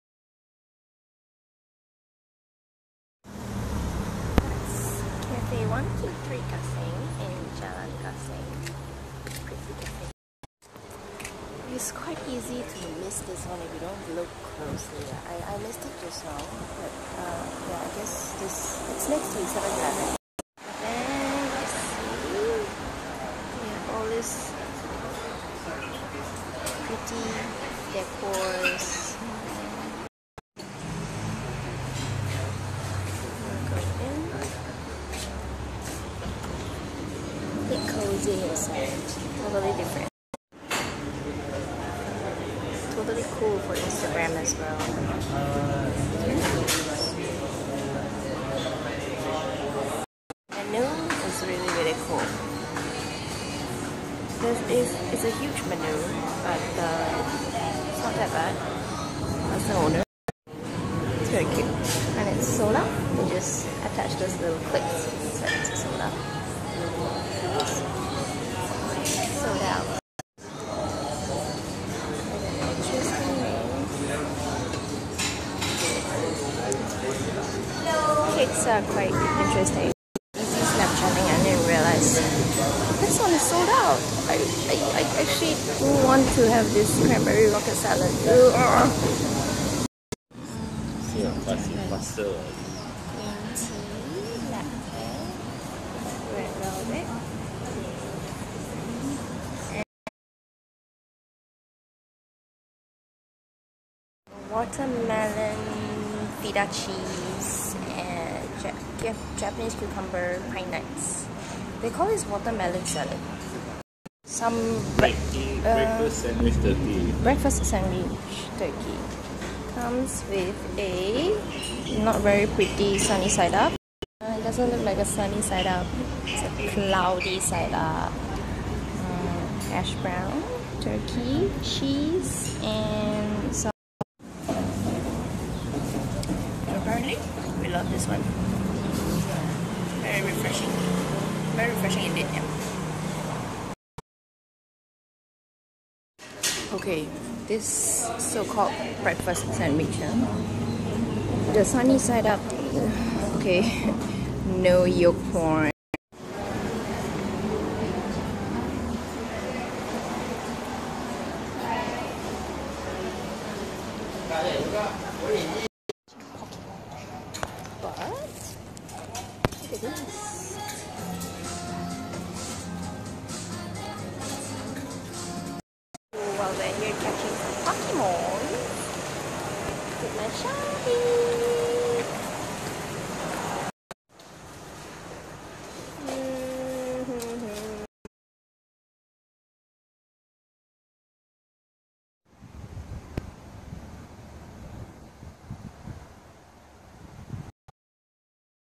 Snapchat 7.8.16- Checked out Gasing 123 cafe at PJ
Spontaneously visited the Gasing 123 Cafe on Jalan Gasing and found somethings that I really like about the place.